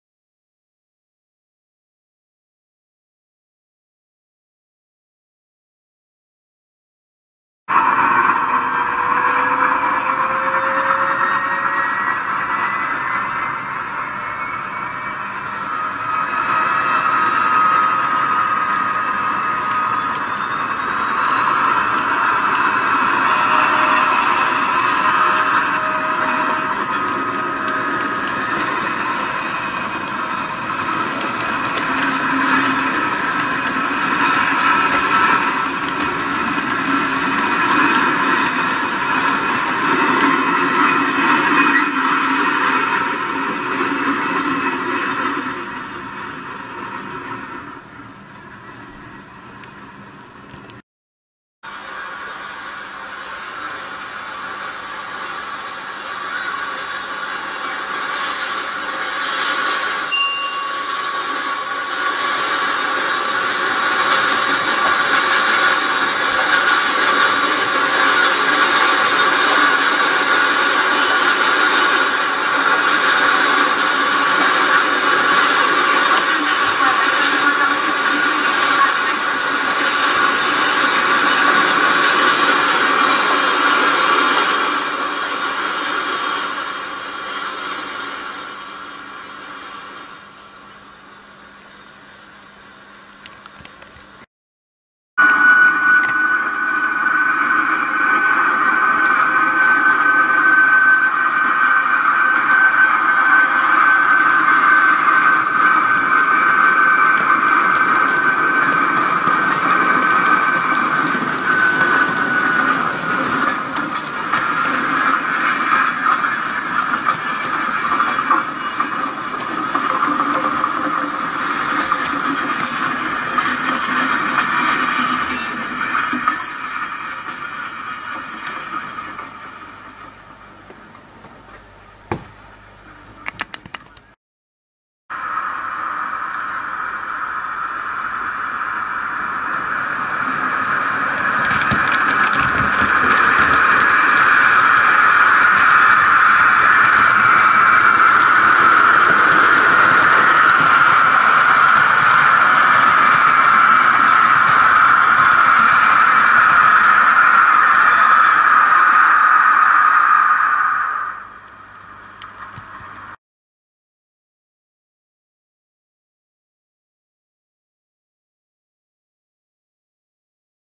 MISTRAL, LA 060 DB, CC 65000 A L'HONNEUR.
La CC MISTRAL en tête d'une rame de voitures OCEM à rivets apparents. Composition réaliste telle qu'il était possible de la voir dans les années 60 autour de LA ROCHELLE.